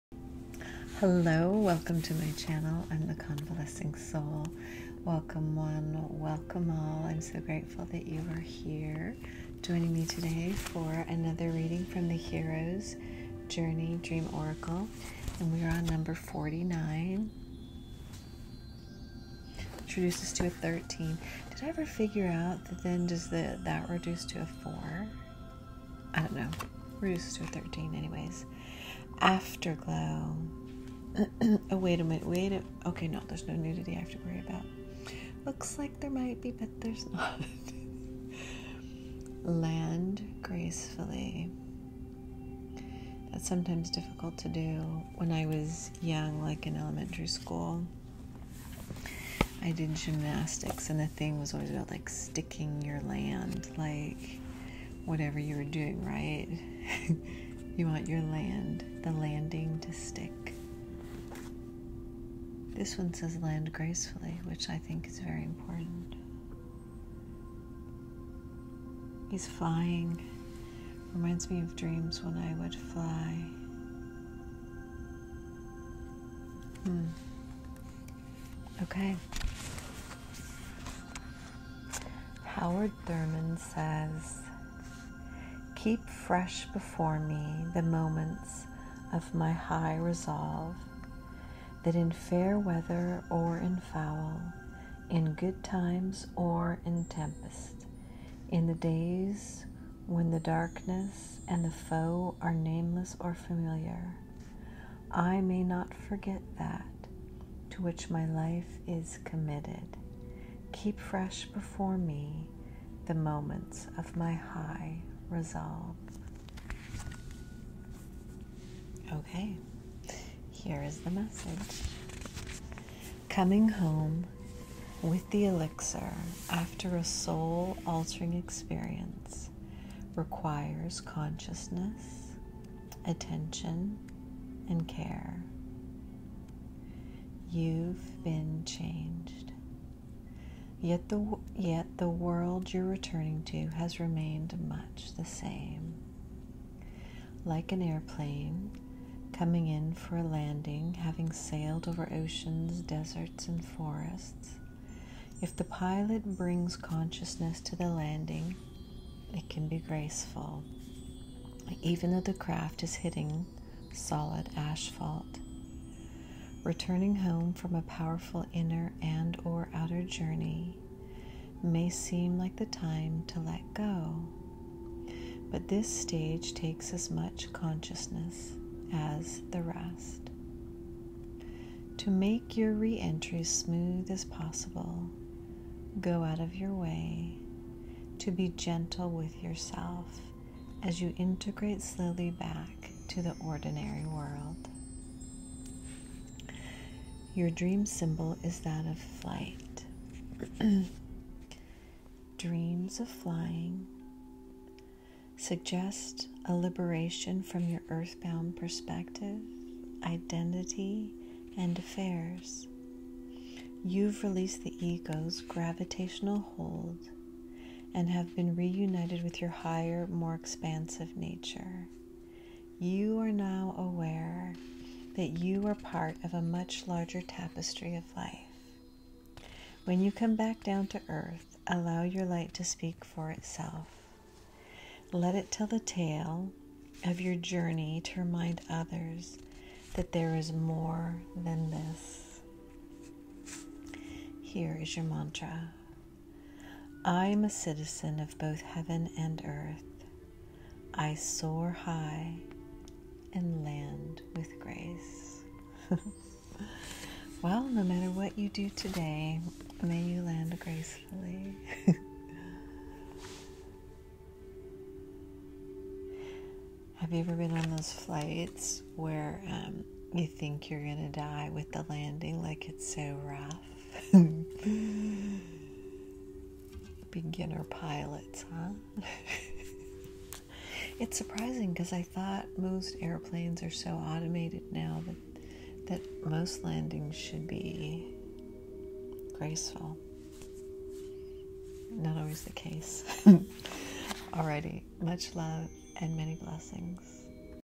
THE HERO'S JOURNEY DREAM ORACLE #49 - AFTER-GLOW
THE PRIVILEGE OF A LIFETIME IS BEING WHO YOU ARE - JOSEPH CAMPBELL.  THE HEROS JOURNEY DREAM ORACLE BY KELLY SULLIVAN WALDEN ENCOURAGES US ALL TO DISCARD THE MASK AND FOLLOW OUR BLISS.  BY FOLLOWING OUR NOBLE PATH AND PAYING ATTENTION TO OUR DREAMS WE WILL FIND WHERE WE ARE MEANT TO BE.